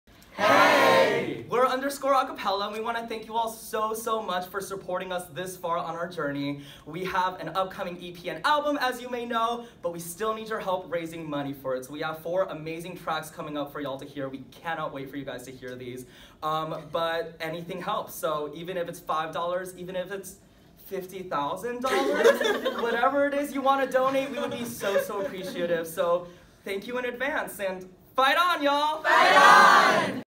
HELP US MAKE AN EP!
Hey Everyone!

We're excited to announce that we're releasing a professionally recorded and edited EP this upcoming year, featuring four of our favorite songs! We want to thank you so much for supporting us this far in our journey, but we still have a ways to go. If you're able to help us out in any way, we'd be incredibly grateful for your support in achieving our financial goals. This EP would mark an incredible milestone for the group, and we can't wait to share all of our hard work with you!

FEATURED MEMBERS ON THE EP:

Taylor Kass
Julia Ehlert
Lexi Staubi
Tara Saha
Daniela Silva
Hannah McCrary
Camille Shrager
Ibum Obu
Alyssa SIbley
Nicolas Zhou
Josh Weadick
Patrick Tran
Ken Murakami
Justin Accardi
Connor Donnelly
Shyam Krish
Conor Hayes
Justin Shin
Noah Gladden
Ivan Tsang